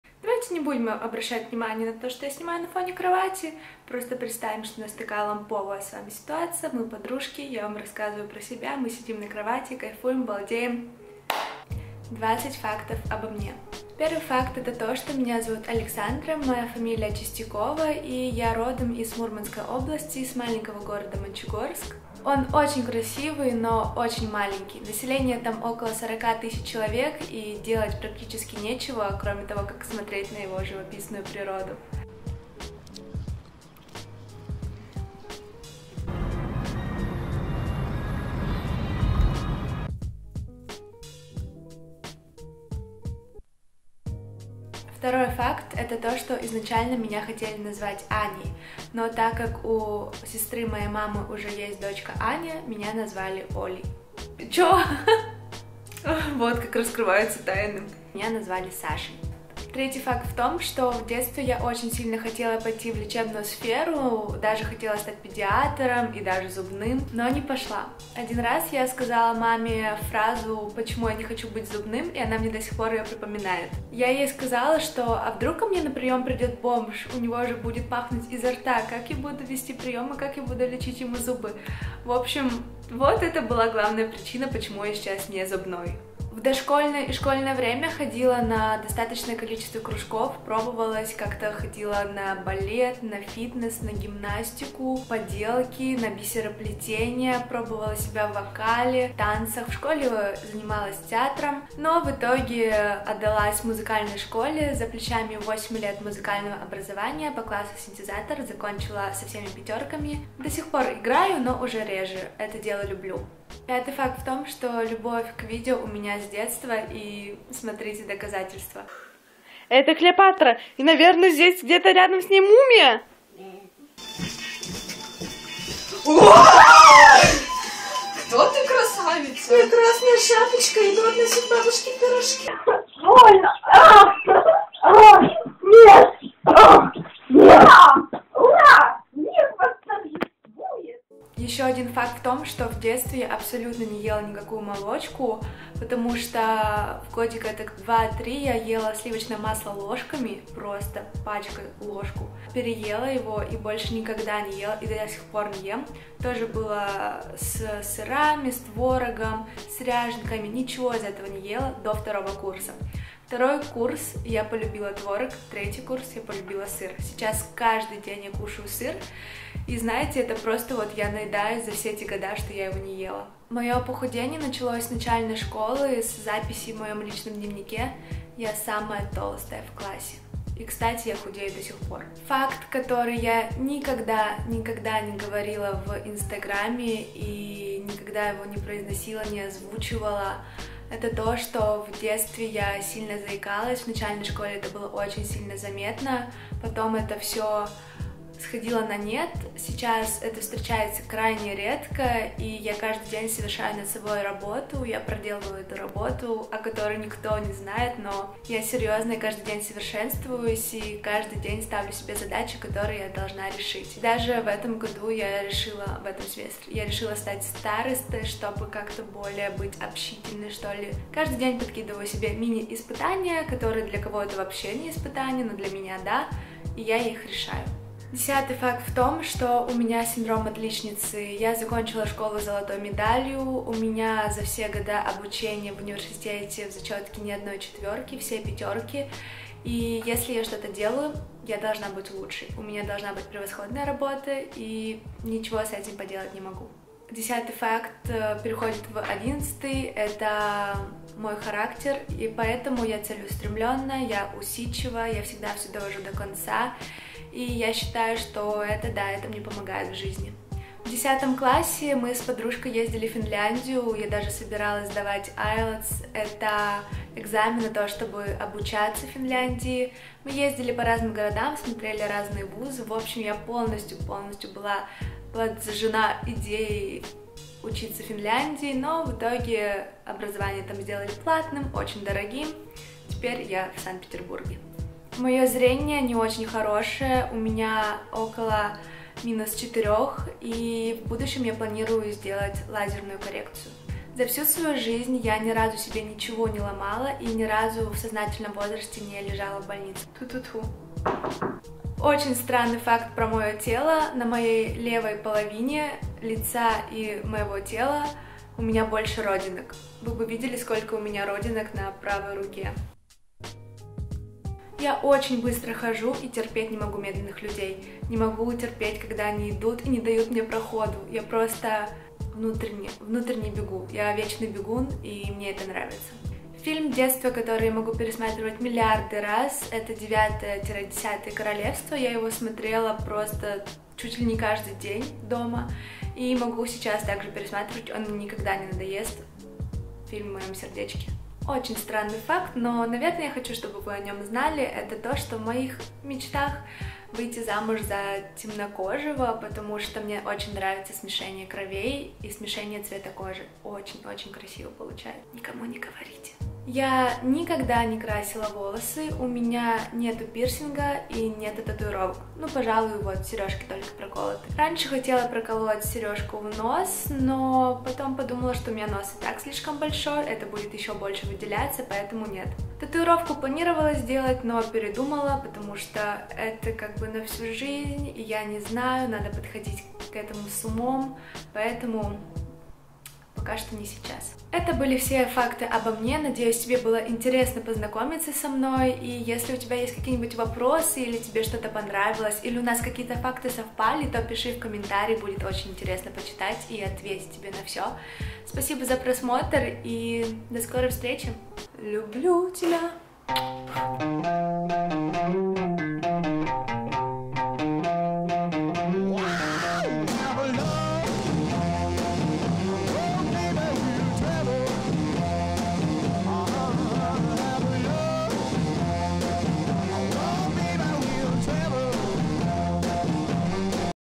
20 фактов обо мне|обучение в финляндии, пирсинг и заикание
приветик,я Саша
на данный момент живу и учусь в Санкт-Петербурге
монтирую видео в movavi
снимаю на сони а5100

если есть вопросы, задавайте в комментариях!